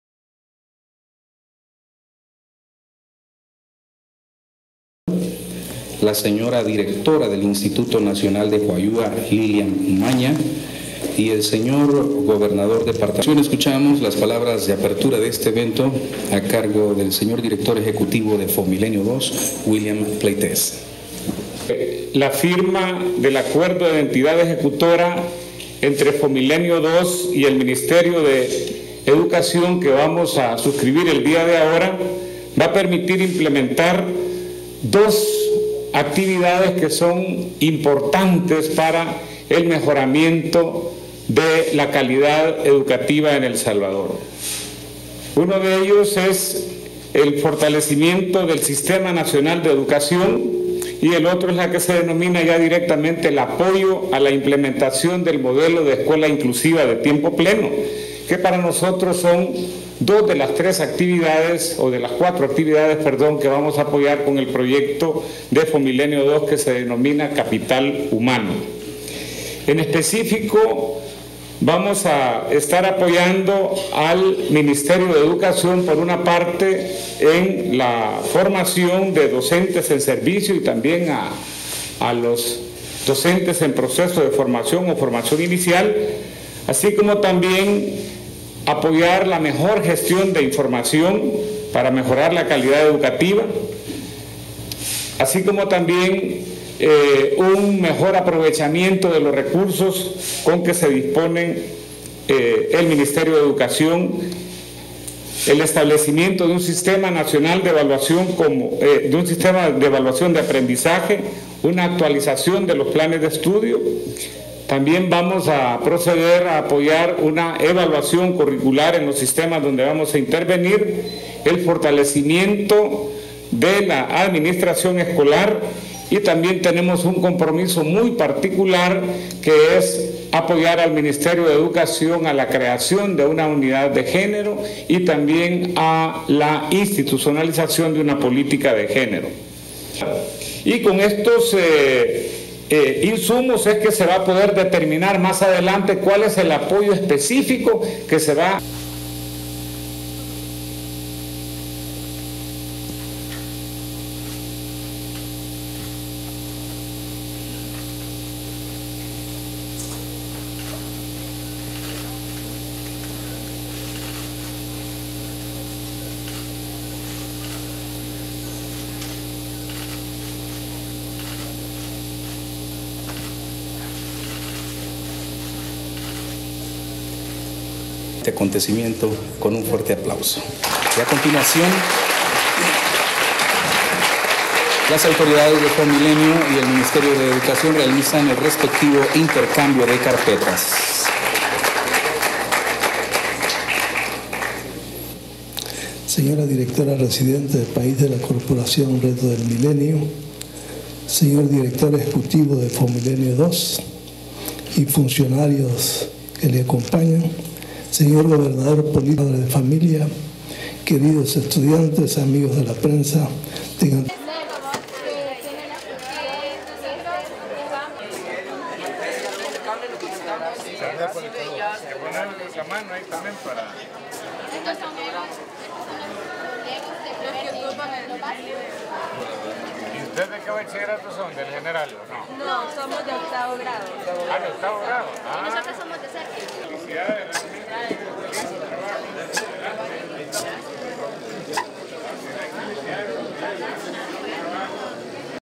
Acuerdo de Entidad Ejecutora entre FOMILENIO II y el Ministerio de Educación 06 de Abril de 2016
Esta mañana, autoridades de FOMILENIO II y del Ministerio de Educación (MINED) firmaron un Acuerdo entre ambas instituciones para implementar la actividad Calidad de la Educación, del proyecto de Capital Humano de FOMILENIO II.

El evento se llevó a cabo en el Instituto Nacional de Juayúa, Sonsonate.
El acuerdo de Entidad Ejecutora apoyará los esfuerzos del MINED en impulsar la formación de docentes en servicio y nuevos docentes, la gestión eficiente de información, tanto para uso interno de la institución como para usuarios externos, el sistema nacional de evaluación de aprendizaje, actualizaciones de planes de estudio y evaluación curricular, el fortalecimiento de la administración escolar y la formulación de una política de género.